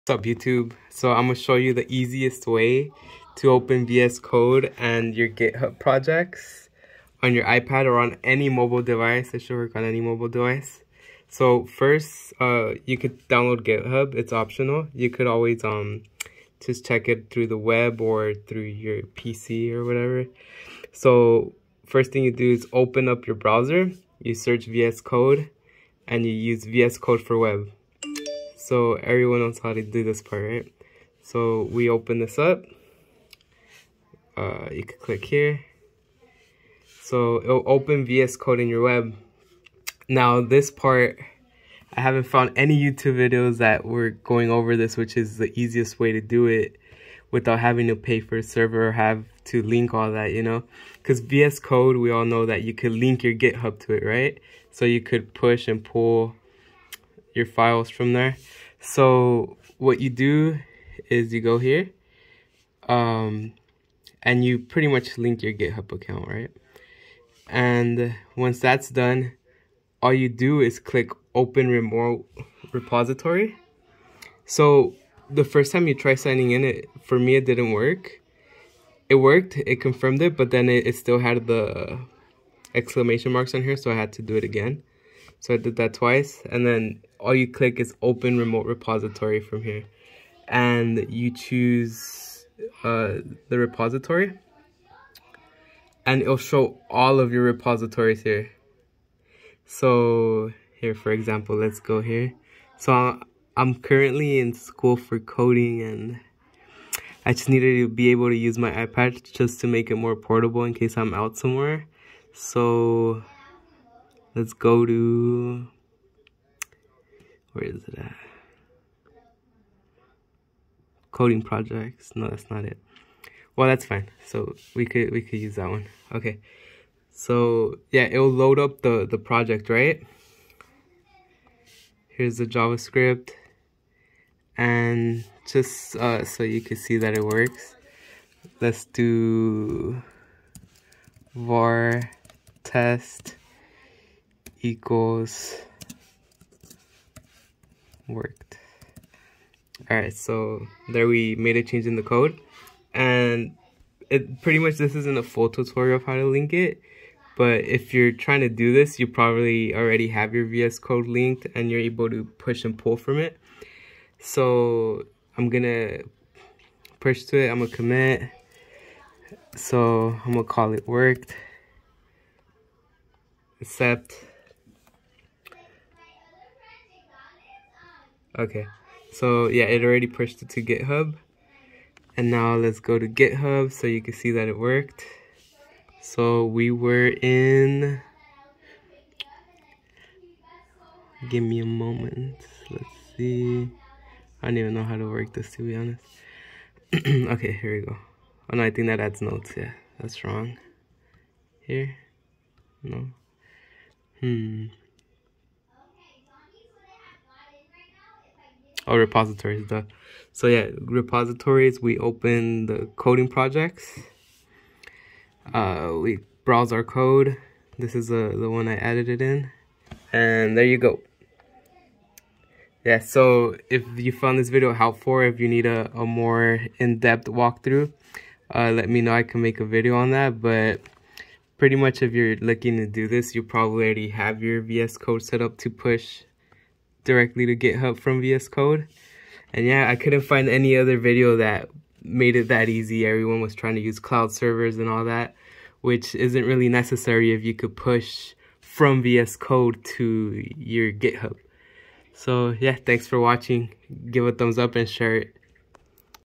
How to use VS code and push to Github on Ipad (or any mobile device) best! easiest! NO CLOUD NEEDED!
tags: How to link your Github with VSCode on your iPad.
In this tutorial, I am explaining how to link your Github account with VSCode no cloud needed!

running on Google Cloud VM using your iPad. All that can be done also on your laptop or PC.